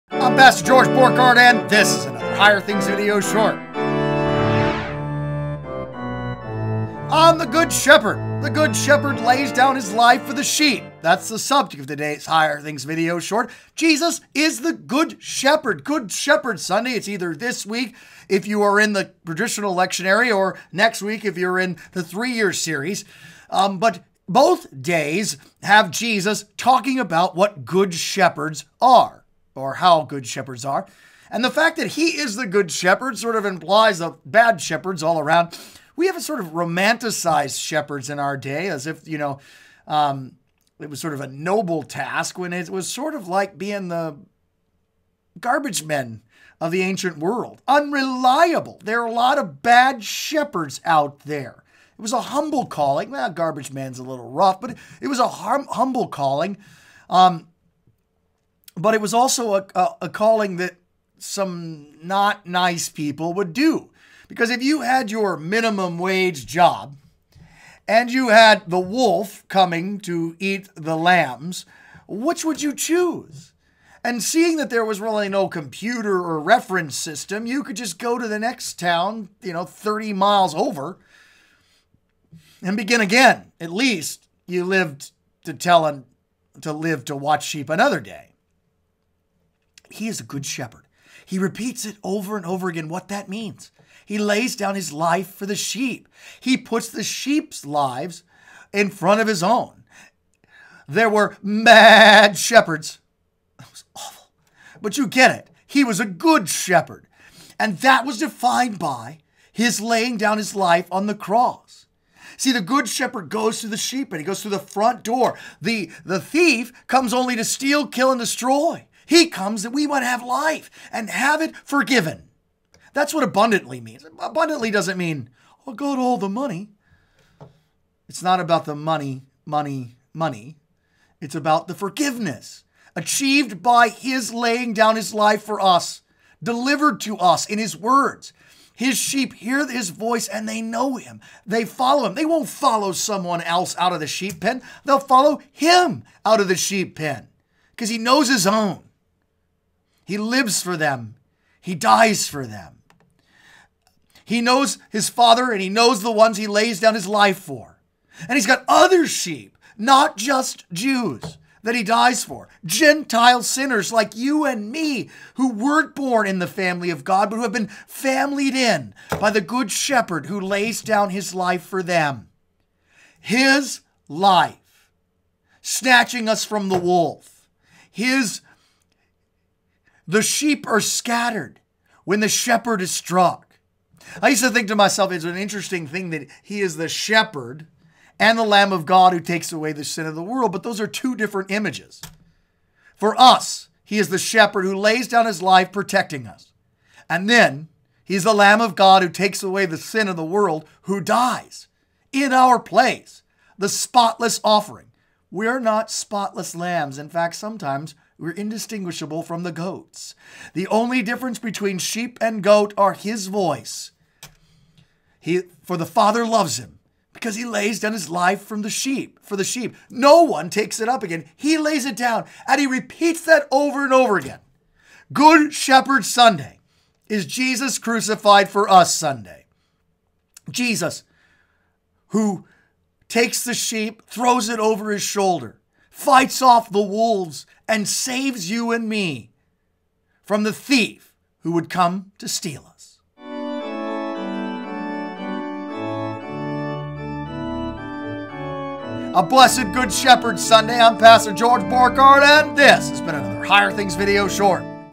Good Shepherd Jesus
Jesus is our Good Shepherd. He lays down His life for His sheep.

If you have questions or topics that you'd like discussed on HTV email them to or send a text to .